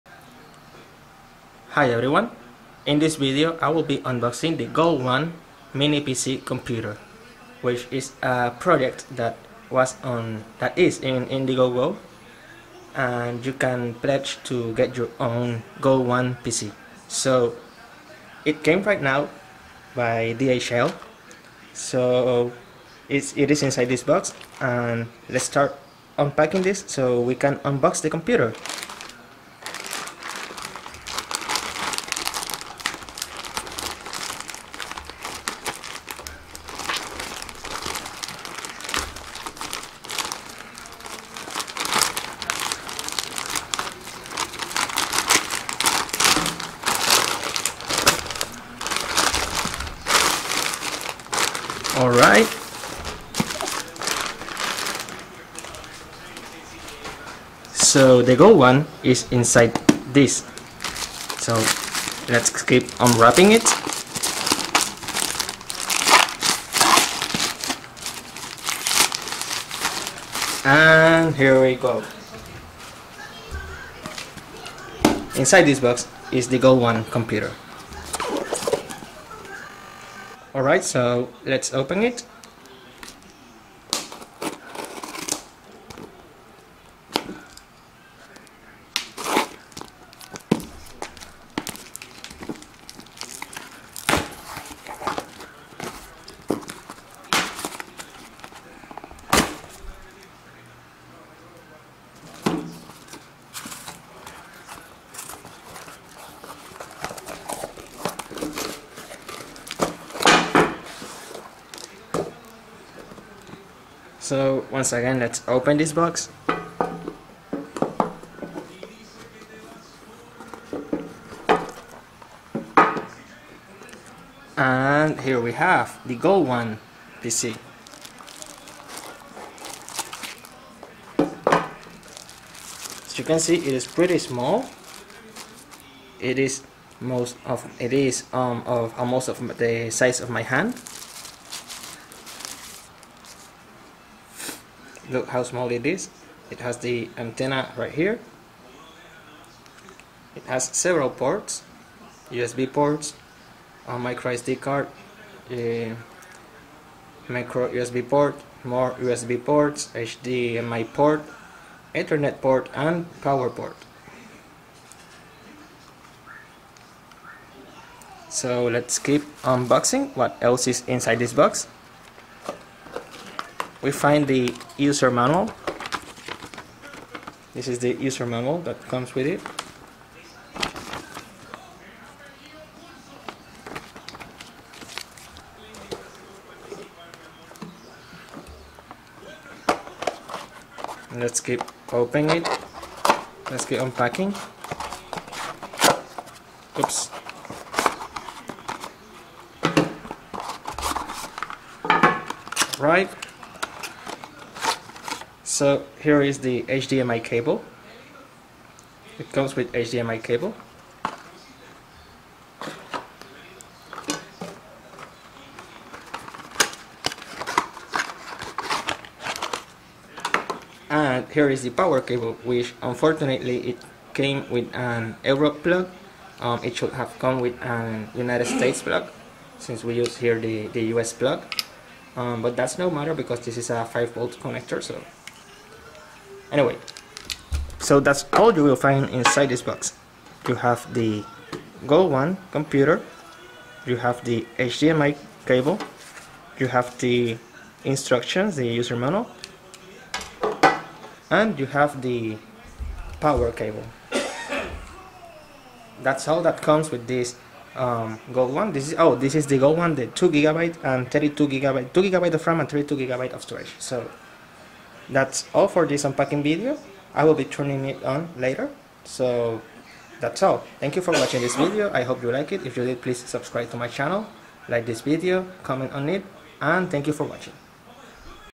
[4K] Unboxing the Gole1 Intel Touch Mini PC
In this video, I will be unboxing the new Gole1 Mini PC, which you can use as a tablet, desktop replacement, a Media Center PC, and more!